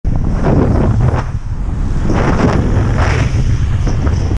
PE54KPA Drives the wrong-way around roundabout, Course Lane, Newburgh, Lancashire
The driver of PE54KPA decides the mini-roundabout is optional, overtaking just metres before it and then proceeds to drive the wrong way around it in the opposing lane.

Who: PE54KPA, PE54 KPA Beige Skoda Fabia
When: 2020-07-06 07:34
Where: Course Lane, Newburgh, Lancashire, WN8 7LA